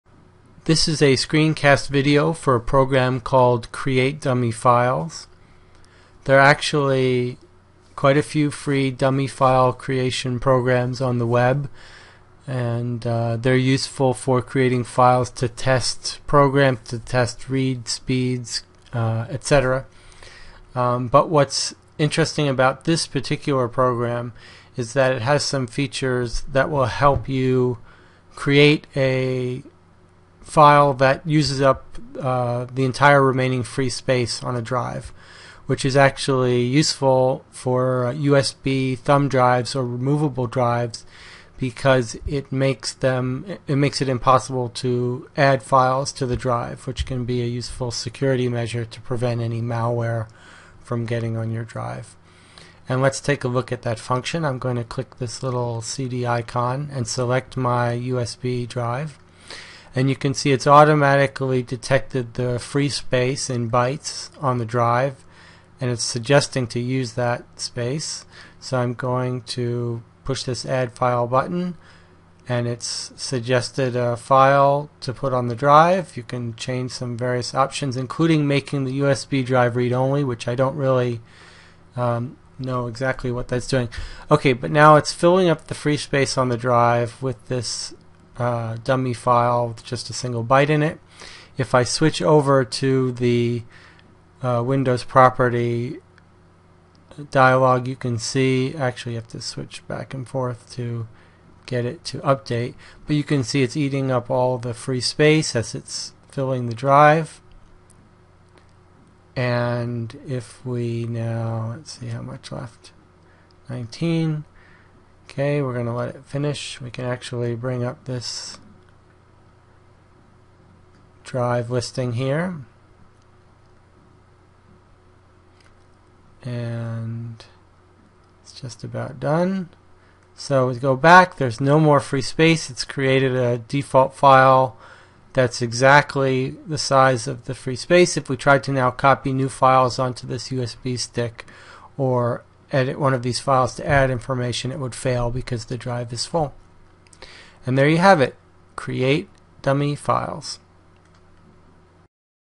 NANY 2010 Entry: Create Dummy File
This is a screencast of Create Dummy File ), which was an entry in NANY 2010

This software is to make files of variable sizes, which do nothing. It uses can be to test disk, Internet speed or to fill a free space on a USB disk. Once the USB is full, with no space left to write, a virus wont be able to write to the USB disk.

, which is the Create Dummy File NANY Entry page.